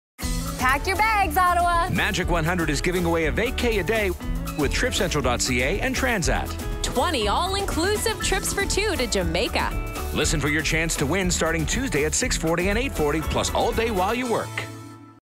STARTING TUESDAY MAJIC 100 IS GIVING AWAY TRIPS FOR 2 TO JAMAICA!
MAJIC 100 HAS THE MOST WINNING IN OTTAWA!  STARTING TUESDAY WE’RE GIVING AWAY TRIPS TO JAMAICA, EVERY WEEKDAY, FROM TRIPCENTRAL.CA!
YOU CAN WIN SEVEN AMAZING ALL-INCLUSIVE NIGHTS AT THE GRAN BAHIA PRINCIPE JAMAICA
LISTEN TO WIN AT 6:40 AND 8:40 PLUS ALL DAY WHILE YOU WORK